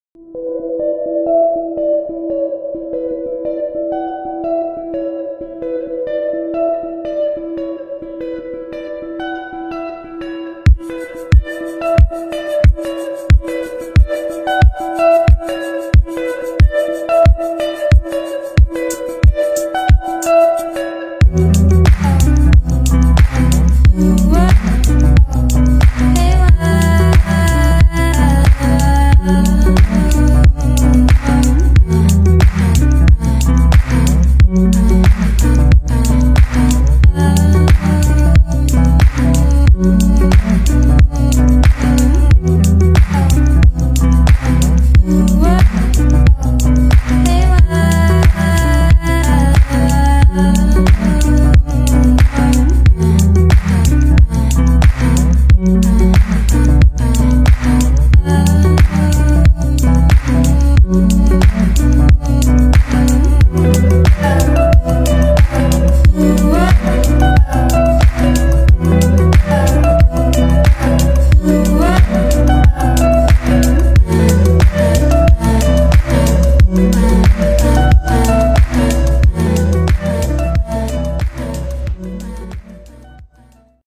Spocky et sa GoPro
Spocky arrive toujours a ressortir des images GoPro avec un angle atypique !